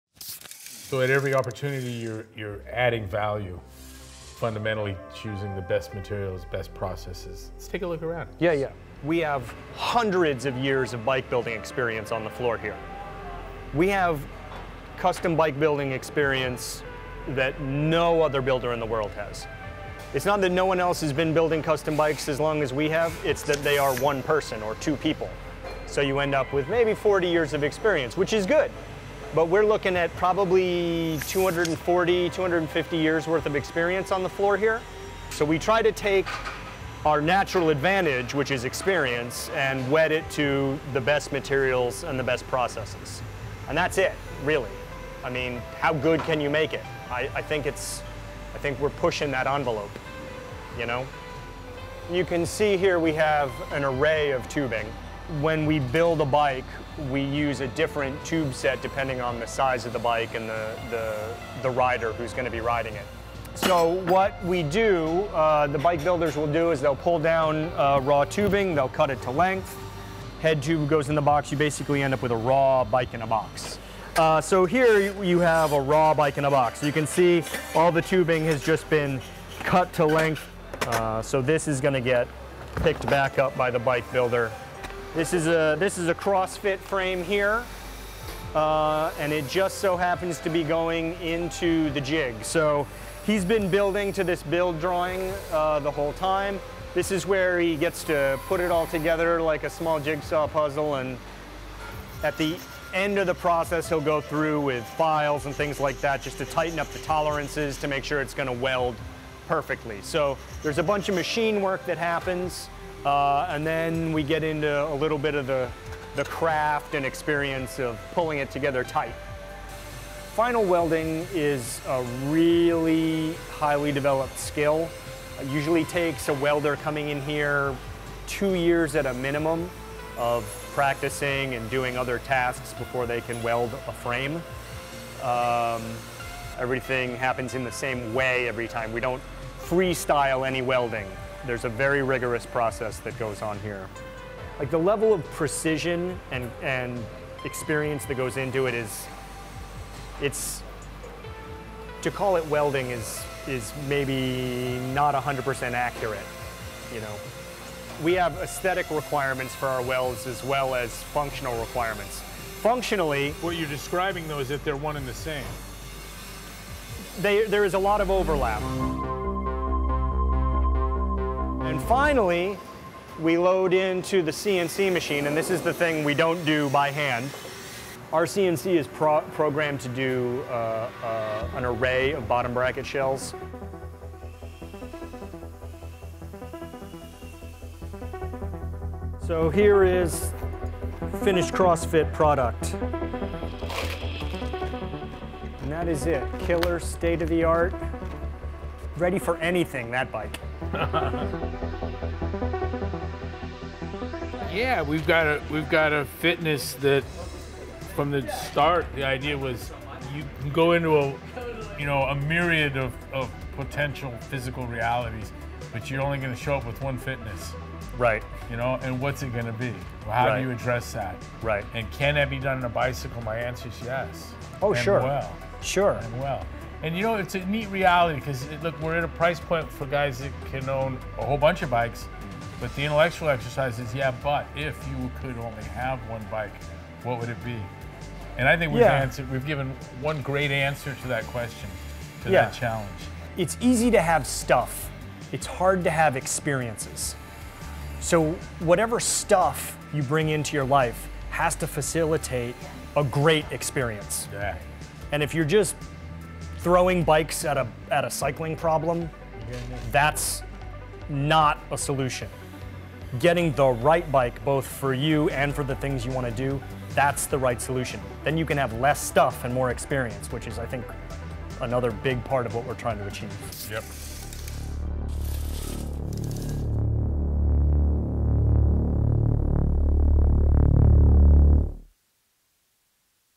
Seven Cycles and the CrossFit Bike
CrossFit has teamed up with Seven Cycles in Boston, Massachusetts, to create the official CrossFit bike. CrossFit Founder Greg Glassman visited Seven Cycles in February to get a glimpse of the production process and talk through the philosophy behind the bike.

According to John Lewis, an account executive at Seven Cycles, the Boston-based company is “the top maker of custom bikes in the entire world.”

Lewis claims this is because Seven’s cadre of custom bike makers collectively has over 240 years of experience.

“We try to take our natural advantage, which is experience, and wed it to the best materials and the best processes,” Lewis says.

Glassman explains why an alliance between Seven Cycles and CrossFit makes sense.

CrossFit prepares someone “to go through a myriad of potential physical realities,” Glassman says. “Can you do that with a bike? My answer is yes.”